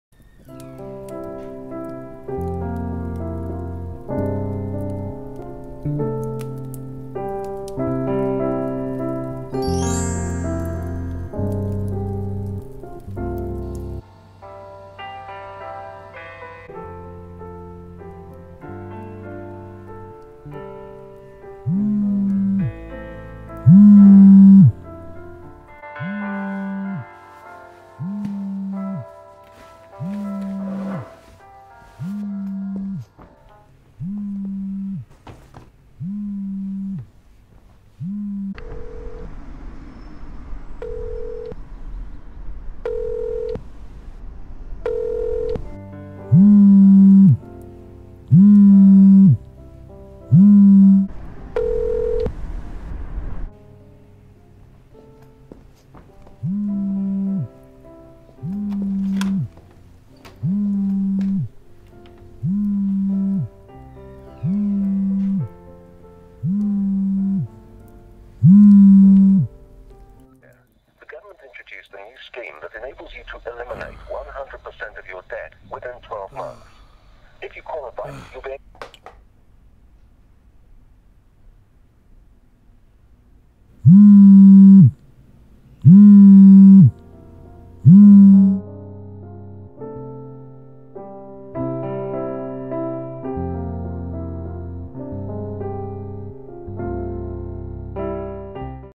No Service Today - Short Christmas Film (2016)
This video was filmed, directed and edited by Callum McMillan. Prepared and shot on December 27th in 2 hours, post-production in 5 hours with imovie '11. This is my first ever short film. All sounds added in post.

Shot on a Panasonic Lumix G6 with the stock lens and a Canon 50mm 1.2 prime lens.